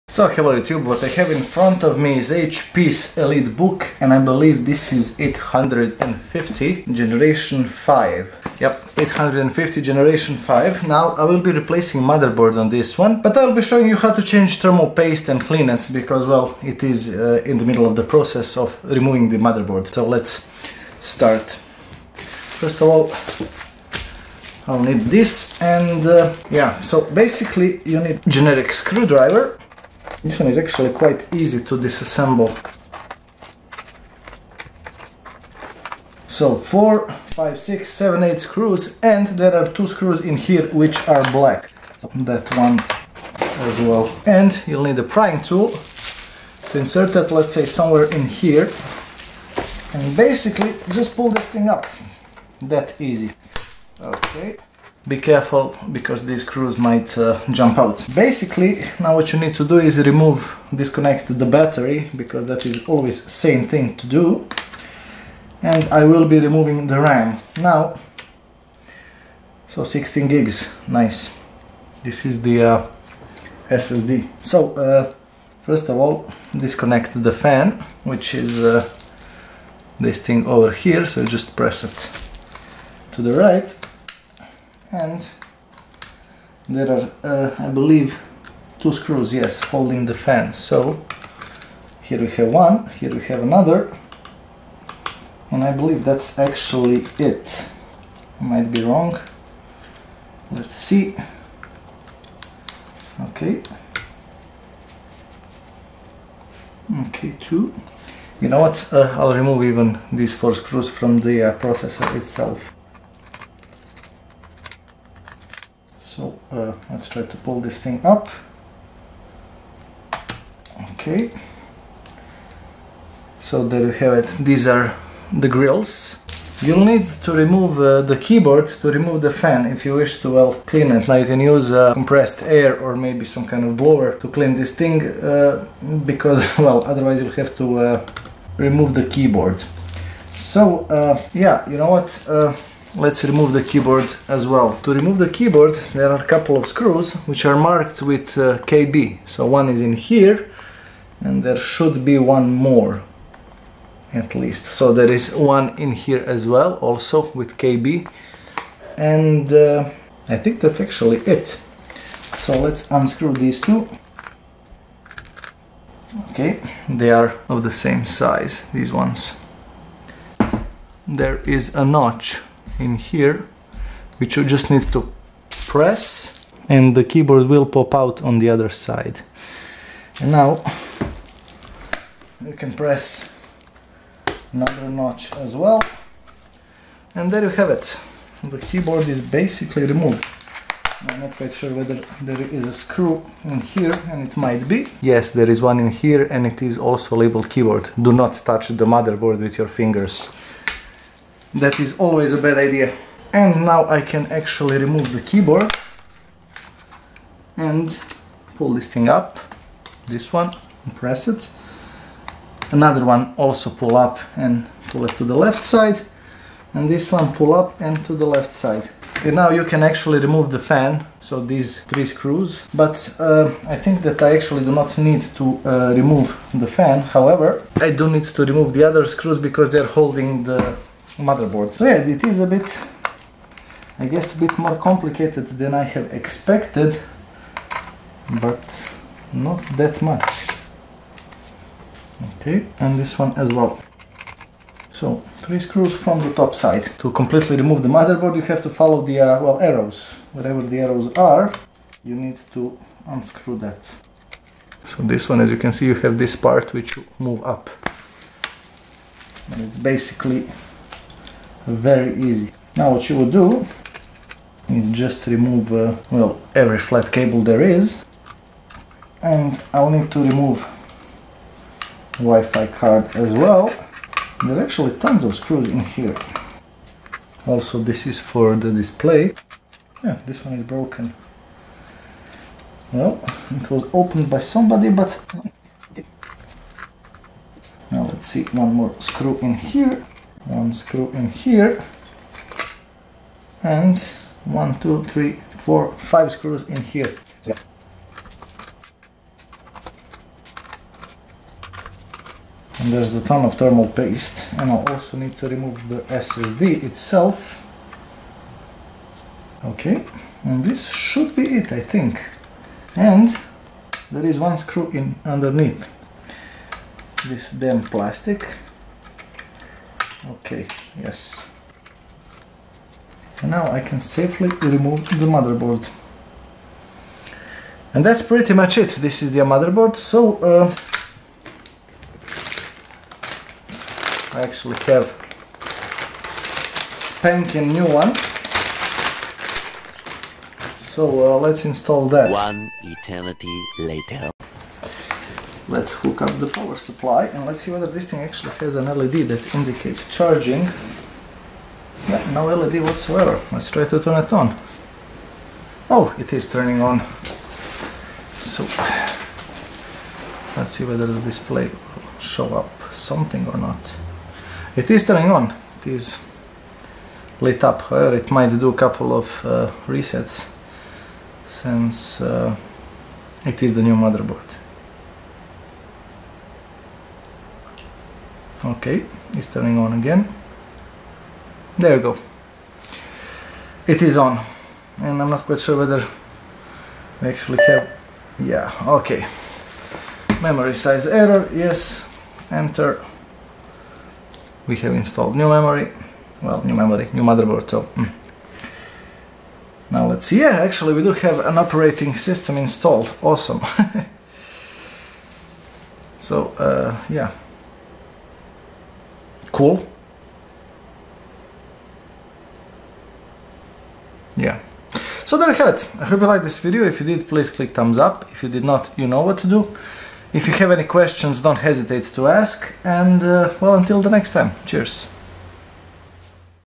EliteBook 850G5 disassembly (motherboard replacement)
I got this for a repair and decided to record video about it. In this video you can see how to:

- clean fan (dust etc)
- upgrade memory
- upgrade SSD
- change motherboard

I did not record assembly since it's pretty much straight forward.

If you have any questions - ask them down below :) I'll try to answer them (if i know)

--- If you find this useful, you are rich as F**K and want to buy me a beer:
BTC: 1P7drxDQALyJDm8rCVSuhEXkiS5LnYFpRr
ETH: 0x21E4cC6BEf890000B1cD52e23F426382d5651B92